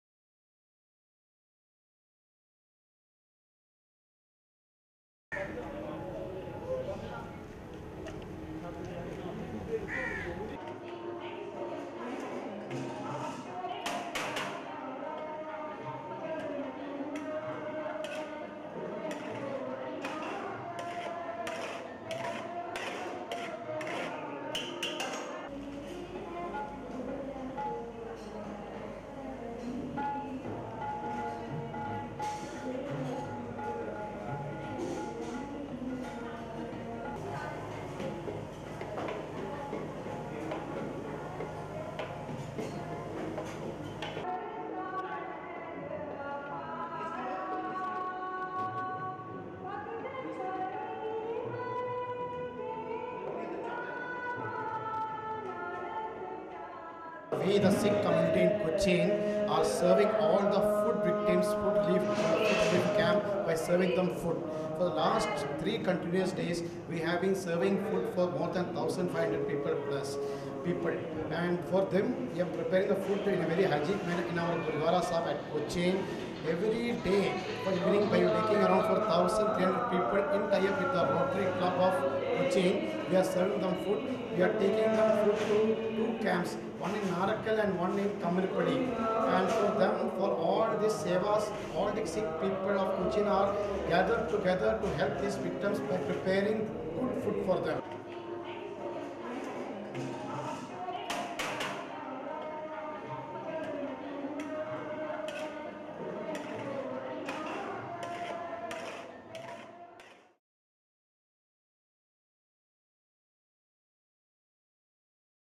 Sikh community in Kochi supplies food to relief camps
Kochi (Kerala), Aug 20 (ANI): The Sikh community in flood-ravaged Kerala’s Kochi district is extending their help the flood victims by preparing food and providing it to relief camps in several parts of Kochi. The coordinator of the Sikh community in Kochi said, “From last three days we are serving food to more than 1000 people.